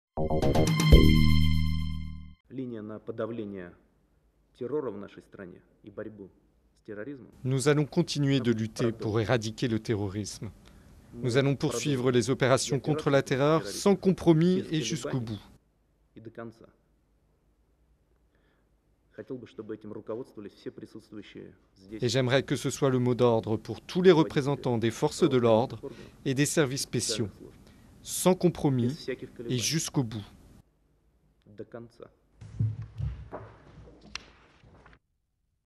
Attentats à Moscou: déclaration de Dmitri Medvedev
Trente-huit personnes, en plus de deux femmes kamikazes, ont été tuées dans le double attentat suicide commis lundi 29 mars dans le métro de Moscou, selon un nouveau bilan des autorités russes. Le président russe Dmitri Medvedev a affirmé que la lutte contre le terrorisme était une priorité absolue et qu'elle allait se poursuivre "sans compromis". Extrait de sa déclaration. Durée 36s